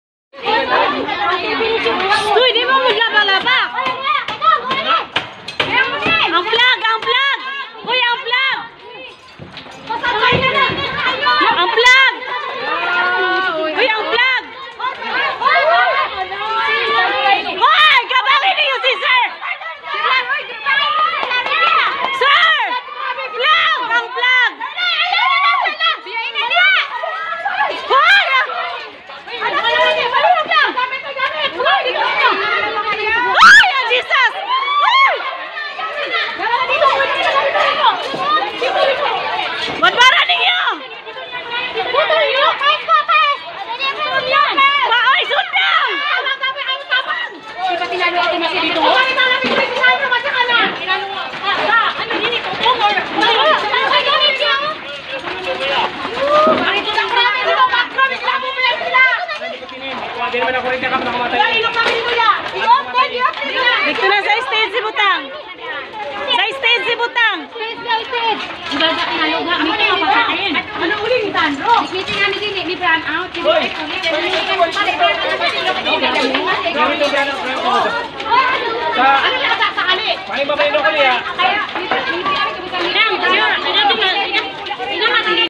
Panoorin: 4 Classrooms Nasunog sa Barangay Bato, Sindangan, Zamboanga del Norte
Panoorin ang mga huling part ng video parang ang CREEPY.